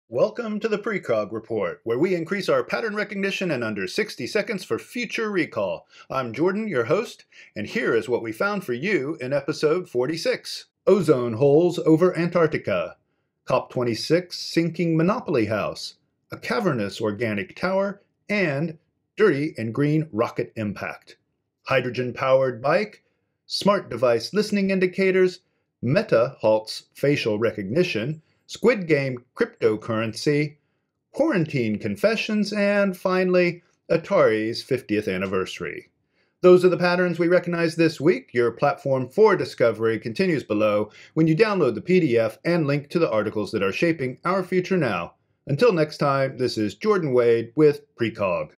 PRECOG ep46: ozone, COP26, hydrogen, Meta, Facial Recognition, Squid Games, Cryptocurrency, Atari
Increase your Pattern Recognition in under 14 seconds with...

PRECOG REPORT ep46: Ozone holes over Antarctica, COP26 sinking Monopoly house, a cavernous organic tower, dirty and green rocket impact. Hydrogen-powered bike, smart device listening indicators, Meta halts Facial Recognition, Squid Game Cryptocurrency, quarantine confessions and Atari 50th Anniversary.

(remember: download pdf. click images. read stories.)

PDF subscription and archive link on homepage

Feed your future